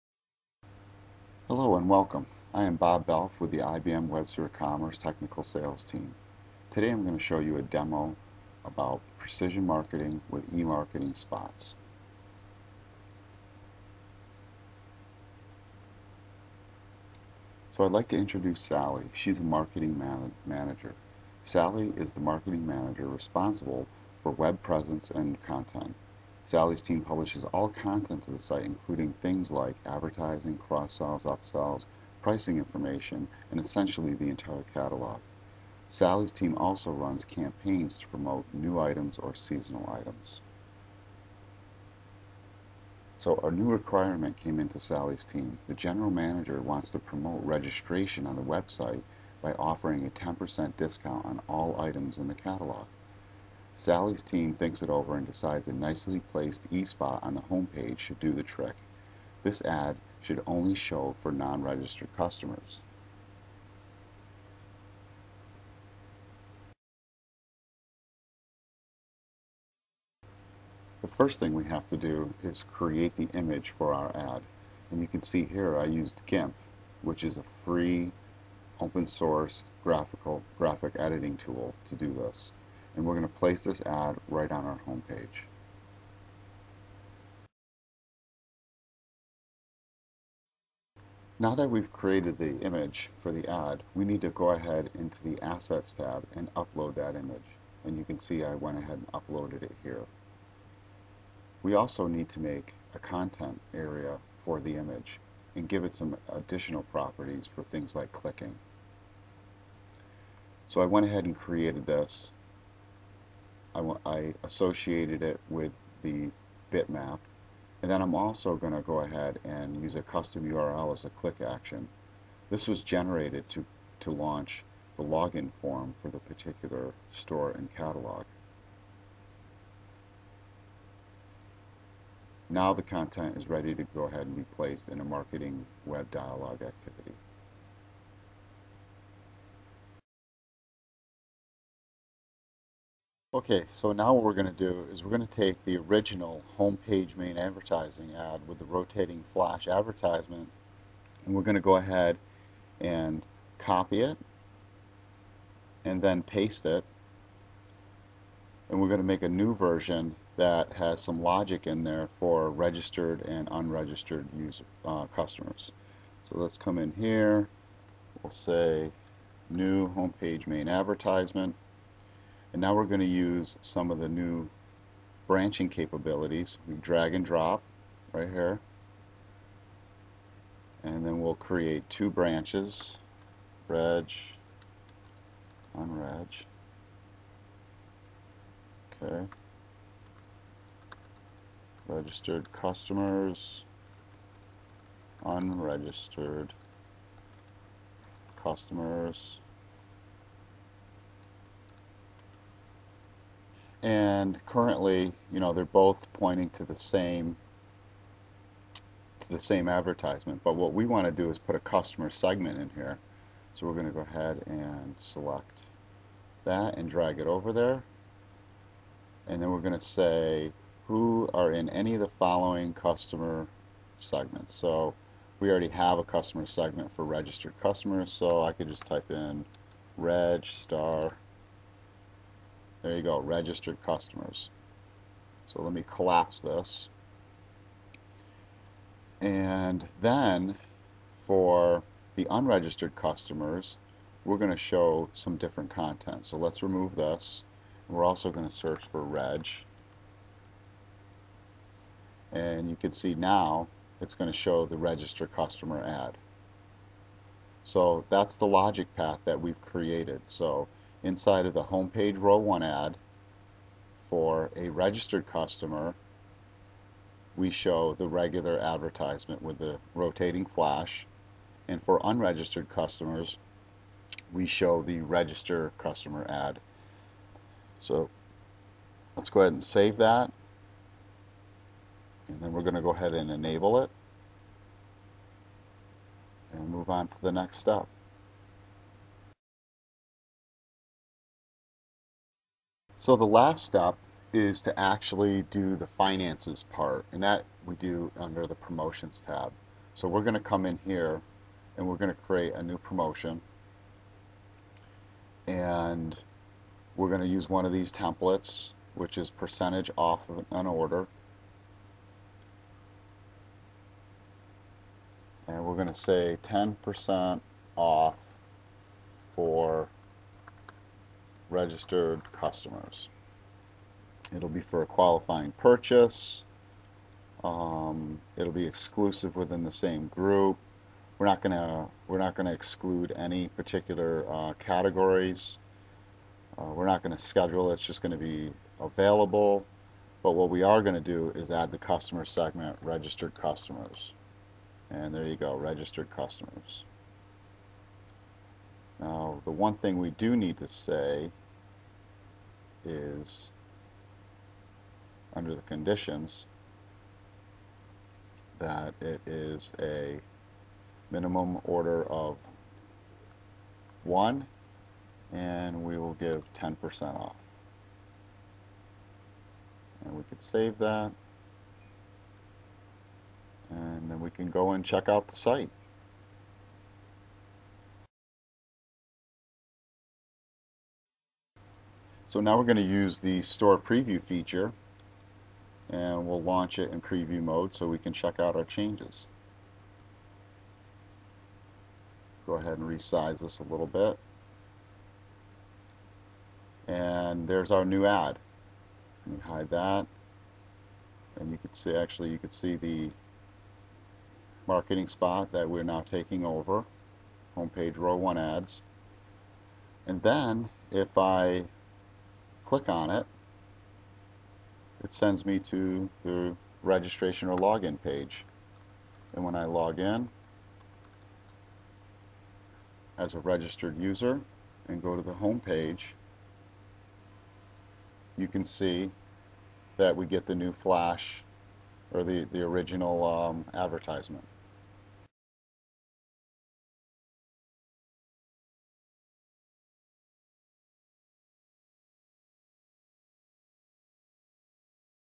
eSpots and Precision Marketing with WebSphere Commerce
This video shows the cool visual editor in the IBM Management Center, which is the administration tool used for WebSphere Commerce. I create a new web activity and promotion to get people to register on the site by offering 10% off all items in the store for registered customers; the video walks you through all of the steps.